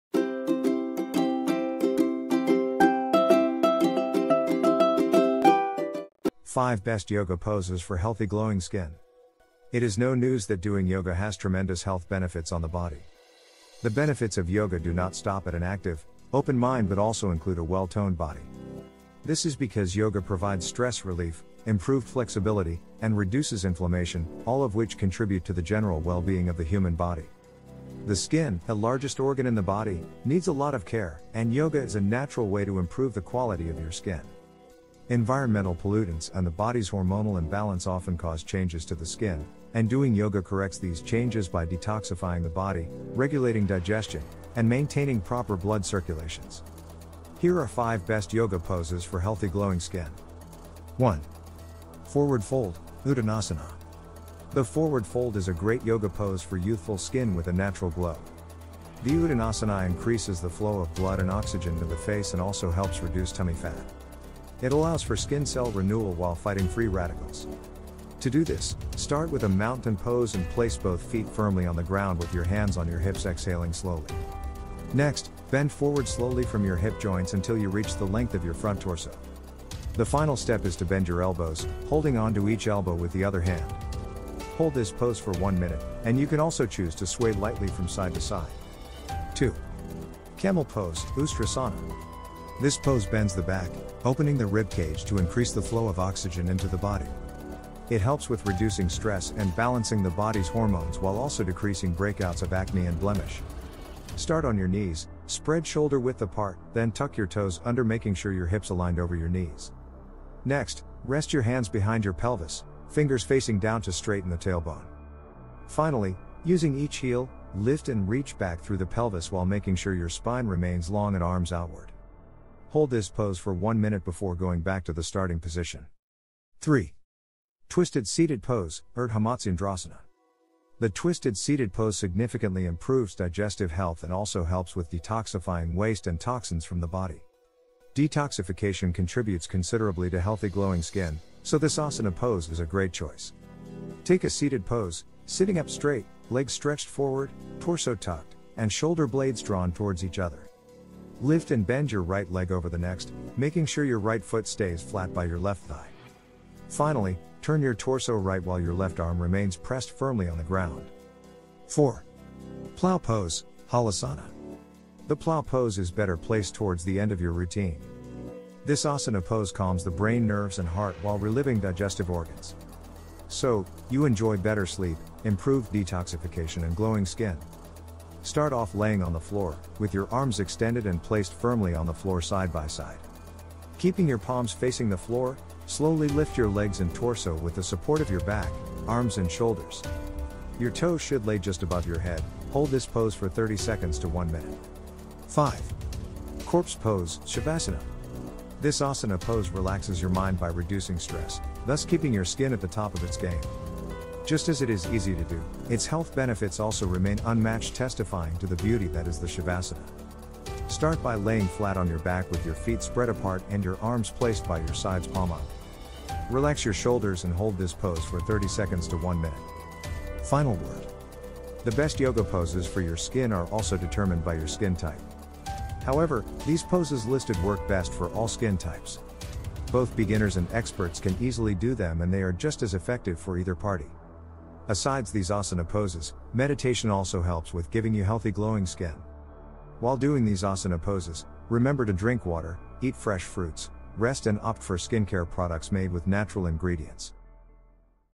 5 Best Yoga Poses For Healthy Glowing Skin
This is a guide you'll learn 5 Best Yoga Poses For Healthy Glowing Skin

Timestamps
00:51 Forward Fold (Uttanasana)
01:29 Camel Pose (Ustrasana)
02:06 Twisted Seated Pose (Ardha Matsyendrasana)
02:42 Plough Pose (Halasana)
03:15 Corpse Pose (Savasana)
03:41 Final Word

★☆★ RESOURCES: ★☆★

-- LINKS –
📕 Download our FREE Ebooks and Guides! 📕

► If you need help growing your business check out my Service Page ➤➤➤